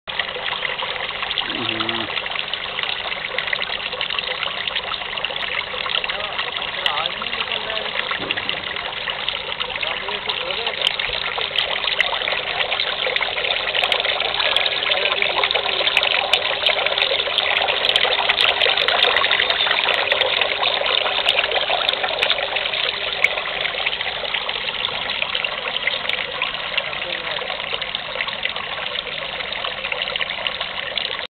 Hanging Fountain Running
Hanging Fountain Demo @ KNT Creations India Pvt Ltd Factory in Pune India